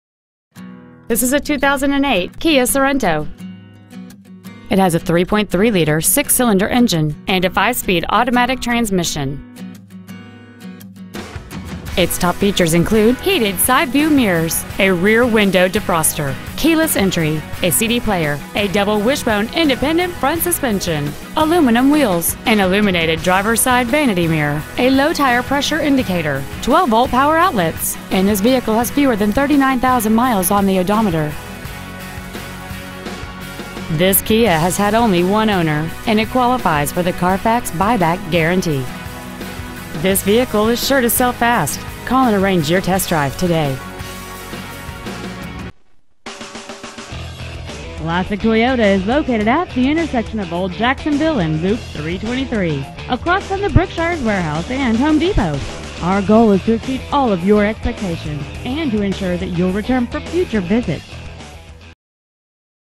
2008 Kia Sorento Tyler TX 75701
Year: 2008
 Make: Kia
 Model: Sorento
 Engine: Gas V6 3.3L/204
 Trans.: Automatic
 Exterior: Silver
 Miles: 38,358
 Interior: Gray

 1717 West SW Loop 323

 Preowned 2008 Kia Sorento Tyler TX